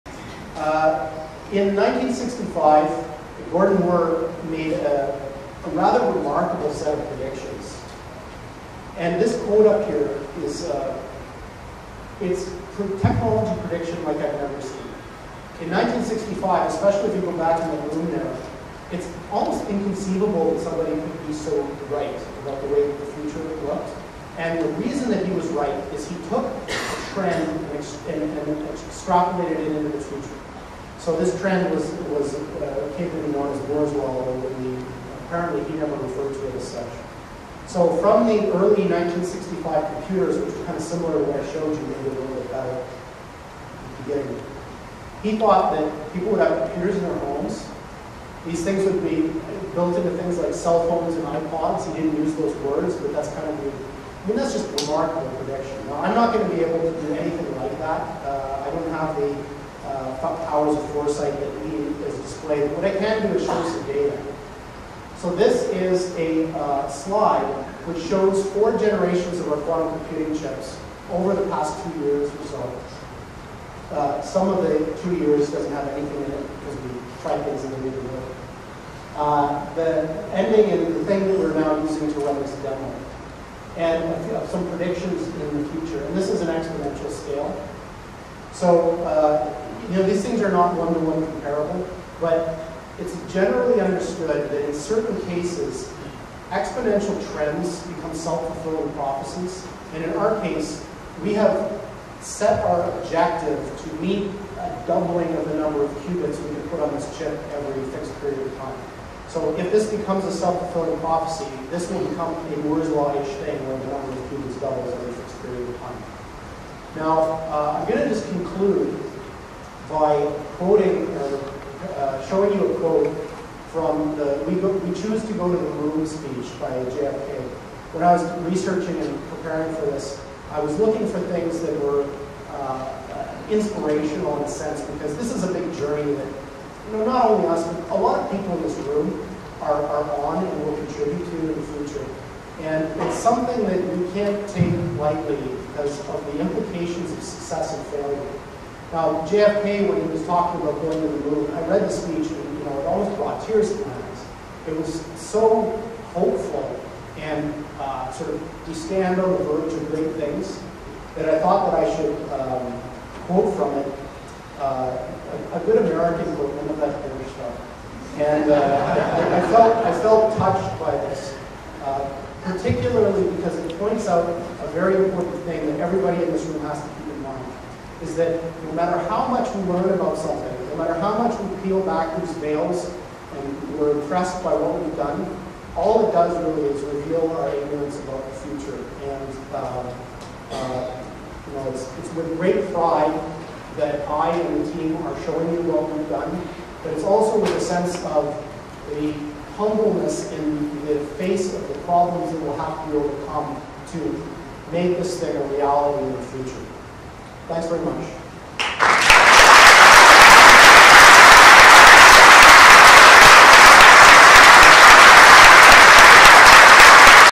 D:Wave's Dr. Geordie Rose part 3 Q.quantum singularity computer
End of the doctor's speech
Interesting presentation.
Here is some video of the D:Wave
quantum computer running an application.
.  A quantum computer is a device for computation that makes direct use of quantum mechanical phenomena, such as superposition and entanglement
 quantum singularity computer qubit Luxvibes.com